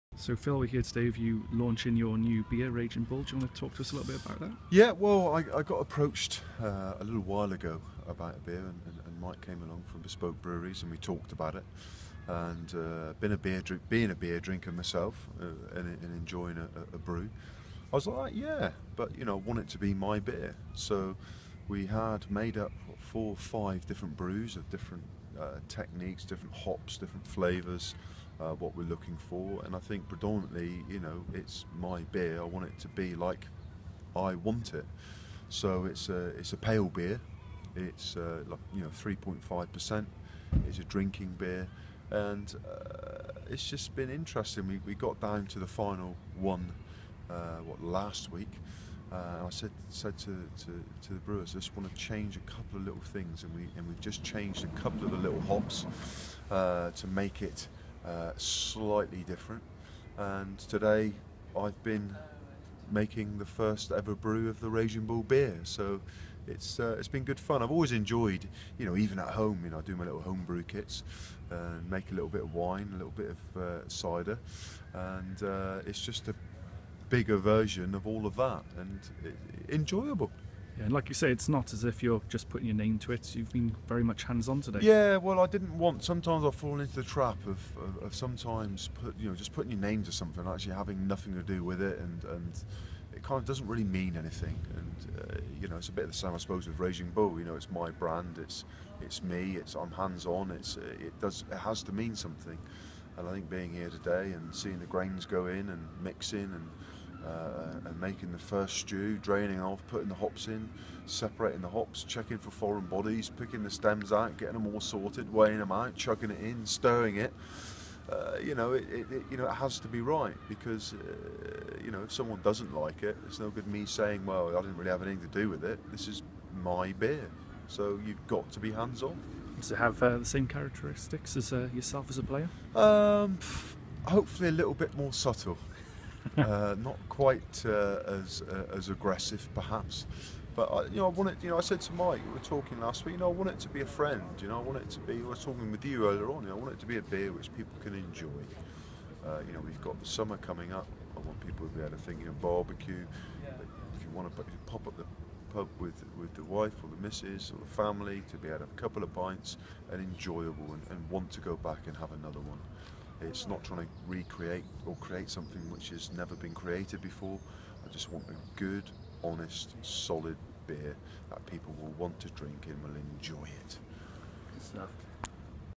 Phil Vickery launches 'Raging Bull Beer' with Bespoke Brewi
Former England and Gloucester prop launches Raging Bull Beer